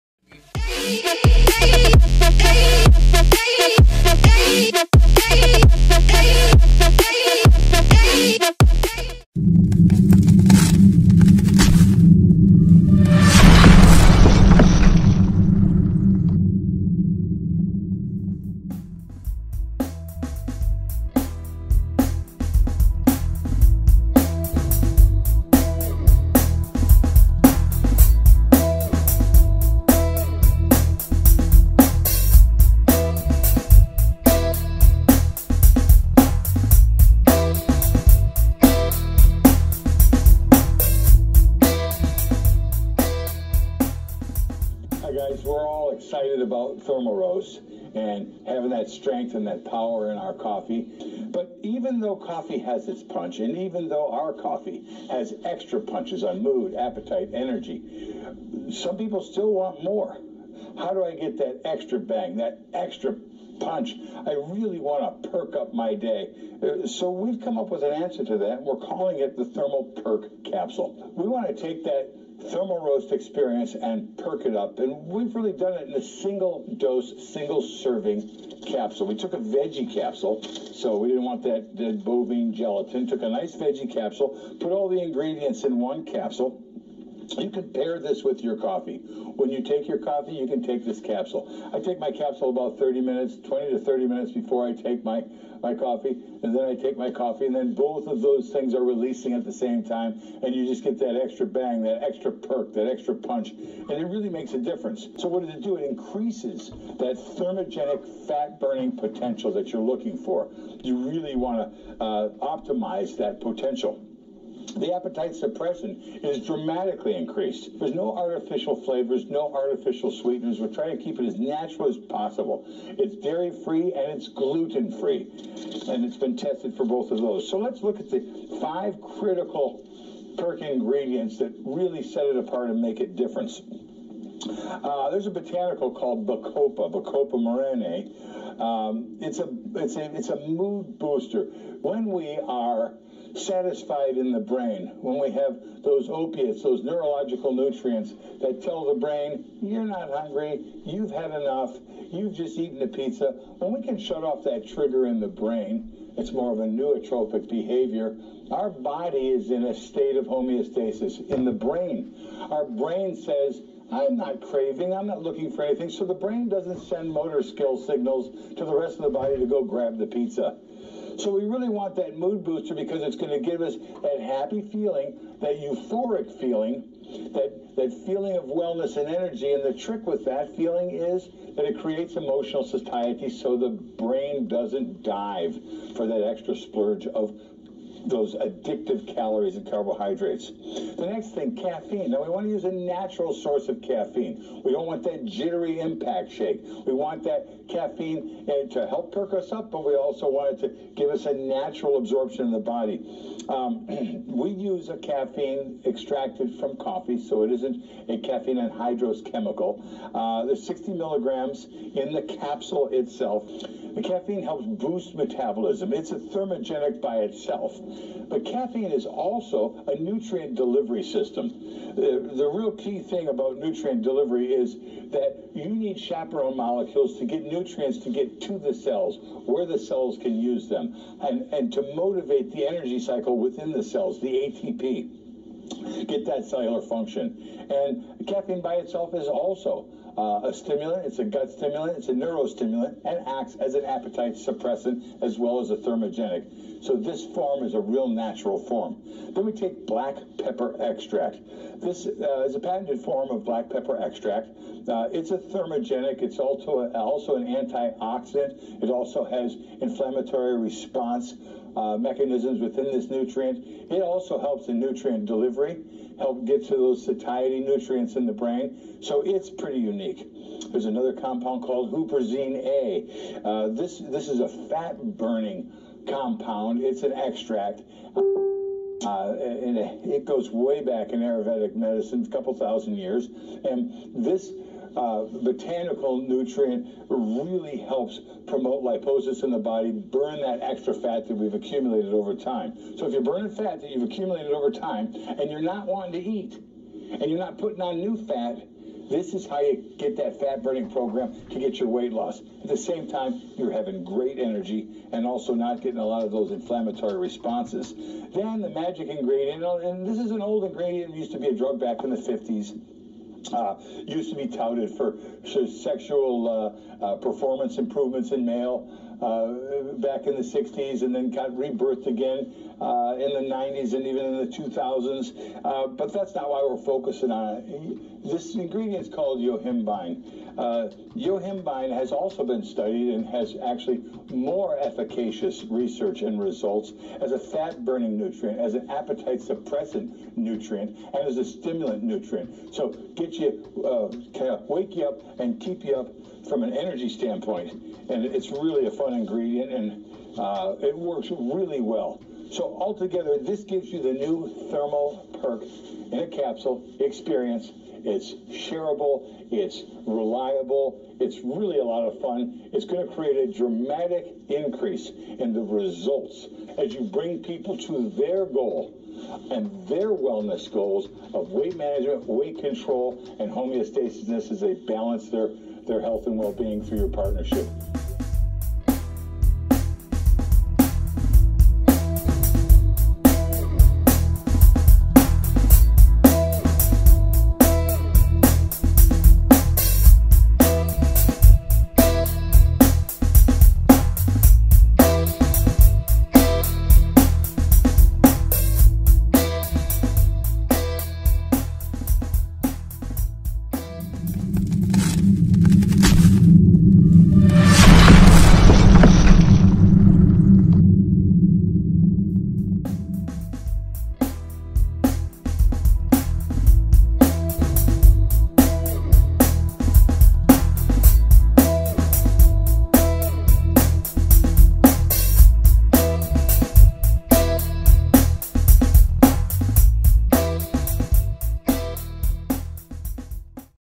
Valentus ThermoPERK New Product | Formulator Kevin Thomas | BG Review
Here's Valentus Product Formulator launching hs new Valentus ThermoPERK weight loss product.

We're all excited about Valentus ThermoROAST and having that power in our coffee. But even though our coffee has extra punches on mood, appetite, and energy ...some people still want more.

So we've come up with an answer that we're calling the thermoPERK veggie capsule.

You could pair this with your coffee. When you take your coffee, you can take this capsule, and you just get that extra bang, that extra perk.

And it really makes a difference.

So what does it do?

It increases that thermogenic fat burning potential that you're looking for. The appetite suppressant is dramatically increased. There are no artificial flavors and no artificial sweeteners. We're trying to keep it as natural as possible.

It's dairy-free, and it's gluten-free. And it's been tested for both of those. So let's look at the five critical ingredients in Valentus ThemoPERK that really set it apart and make a difference.

There's a botanical ingredient in ThermoPERK called Bacopa. Bacopa is a mood booster. When we are satisfied in the brain, when we have those opiates, those neurological nutrients that tell the brain you've had enough.

 It's more of a nootropic behavior. Our body is in a state of homeostasis in the brain. Our brain says I'm not craving, I'm not looking for anything. So the brain doesn't send motor skills signals to the rest of the body to go grab the pizza.

So we really want that mood booster because it's going to give us a happy euphoric feeling. A feeling of wellness and energy.

And the trick with that feeling is that it creates emotional satiety so the brain doesn't dive for that extra splurge of those addictive calories and carbohydrates.

The next ingredient in Valentus ThermoPERK is caffeine.

We want a natural source of caffeine. We don't want that jittery impact shake. We want that caffeine to help cook us up. But we also want it to give us a natural absorption of the body.

We use a caffeine extracted from coffee so it isn't a caffeine anhydrous chemical. There 60 milligrams in the capsule. The caffeine helps boost metabolism. It's a thermogenic by itself. Caffeine is also a nutrient delivery system.

The real key thing about nutrient delivery is that you need chaperone molecules to get nutrients to get to the cells where the cells can use them. And to motivate the energy cycle within the cells, you get that cellular function.

Caffeine by itself is also a stimulant. It's a neuro stimulant and acts as an appetite suppressant as well as a thermogenic. So this is a real natural form.

Next up for Valentus ThermoPERK we take black pepper extract.

This is a patented form of black pepper extract. It's a thermogenic. It's also an anti oxidant. It also has inflammatory response mechanisms within these nutrients. It helps in nutrient delivery, helps get those satiety nutrients in the brain. So it's pretty unique.

There's another compound in ThermoPERK called huperzine A.

This is a fat-burning compound. It's an extract. And it goes way back in Ayurvedic medicine a couple thousand years. And this botanical nutrient really helps promote the body burn that extra fat that we've accumulated over time.

So if you want to burn fat that you've accumulated over time, this is how you do it.

The next ingredient for ThermoPERK is a magic ingredient called Yohimbine that is an older ingredient. It used to be a drug back in the 50s and was touted for sexual performance improvements in males back in the 60s.

Then it made a comeback in the 90s and even in the 2000s. But that's not why we're focusing on Yohimbine. Yohimbine has also been studied and is actually more efficacious as a fat-burning nutrient. It's an appetite suppressant nutrient and a stimulant nutrient.

So altogether, this gives you the new Valentus ThermoPERK in a capsule experience. It's shareable and it's reliable.

It will create a dramatic increase in the results as you bring people to their wellness goals of weight management, weight control, and homeostasis.

Thanks,
Brett Gurney
Team Valentus United Founder
Valentus Leader

My Other Properties

Home Page -
Internet Marketing for Beginners